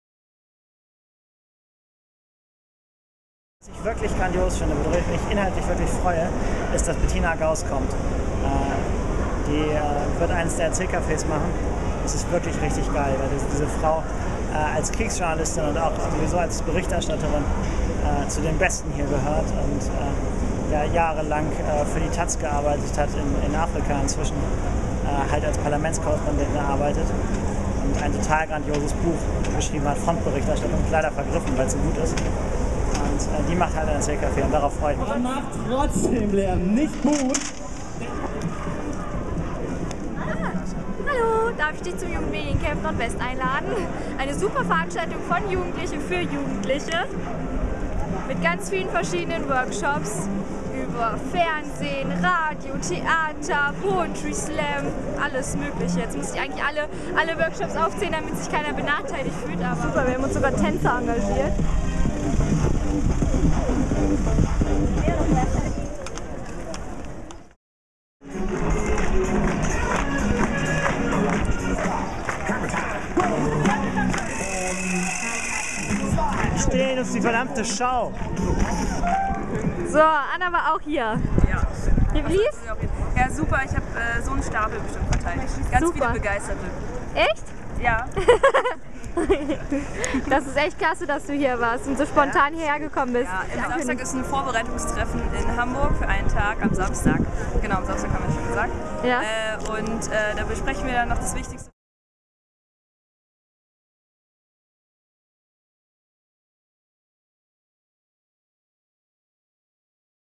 Werbeaktion in Hannover
Vom 01. bis zum 05. Juni verwandelt sich ein Zeltplatz in Loxstedt bei Cuxhaven in eine riesige Redaktion. Rund hundert Jugendliche im Alter von 15 bis 26 Jahren werden in verschiedenen Workshops das Handwerk eines Journalisten, eines Filmemachers, eines Schauspielers oder eines Moderators kennenlernen. Zum neunten Mal findet das JugendMedienCamp NordWest statt und auch in diesem Jahr wird das Konzept „von Jugendlichen für Jugendliche" umgesetzt. Euch erwartet neben coolen Workshops zusätzlich eine spannende Podiumsdiskussion, Erzählcafés und natürlich ein buntes Campleben mit Lagerfeuer, Wasserschlachten und vielem mehr! Beim JugendMedienCamp NordWest können alle Medienbegeisterten ihre Zelte aufschlagen, egal, ob sie schon Erfahrungen in Medienproduktionen gesammelt haben oder zum ersten Mal einen Fuß in die Medienlandschaft setzen!